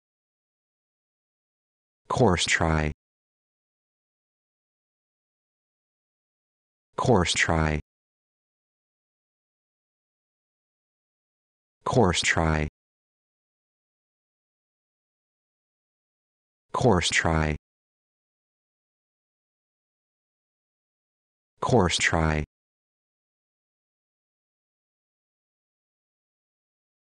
How to Pronounce Corsetry
This video shows you how to pronounce Corsetry